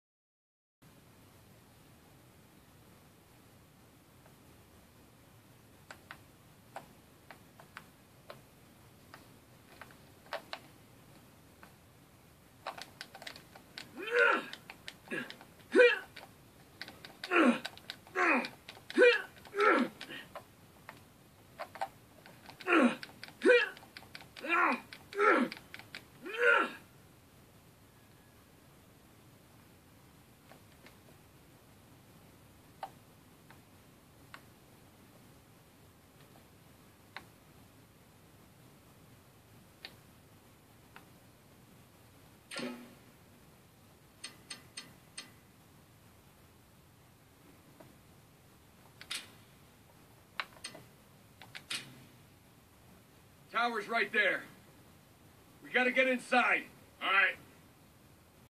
Drake Gets Stuck
Yeah, I don't have a capture card, but I felt like sharing this glitch anyway.

Not sure how it happened; I jumped onto the bricks from a higher ledge and Drake got wedged in.